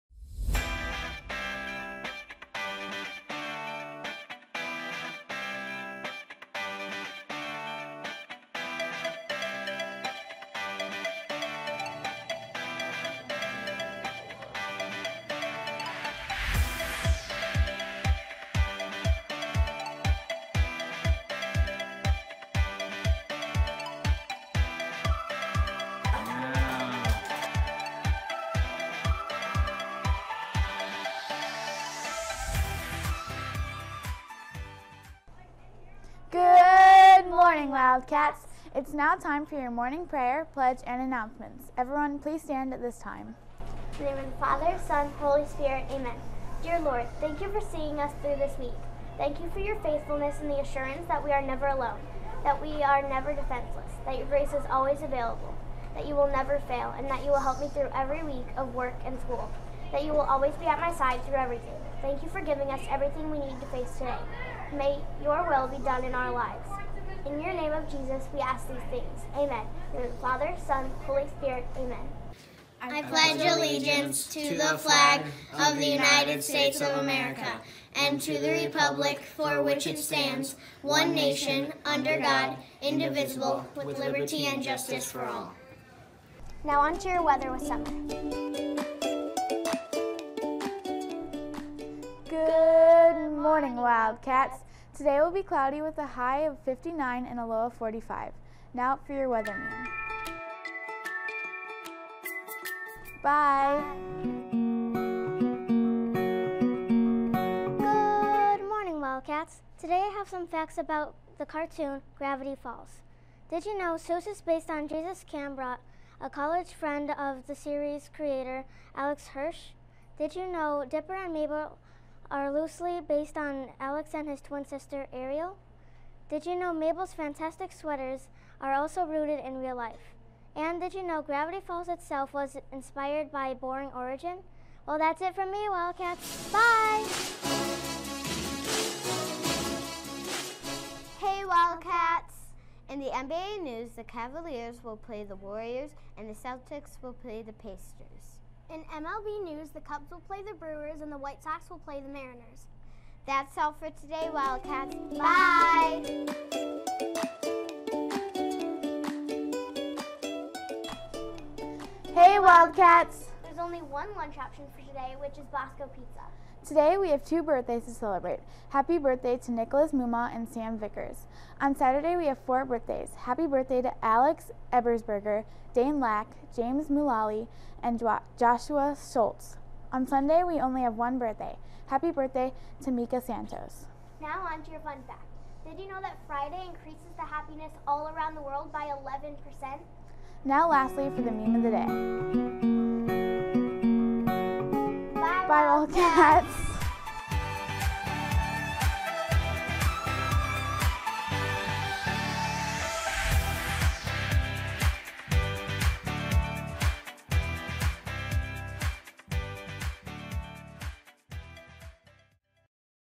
WSMW Wildcat - Spring Season - Crew 2 - Episode 19 - 2019 4 5
MEMES and MEMES, plus a look into the show Gravity Falls in this special edition.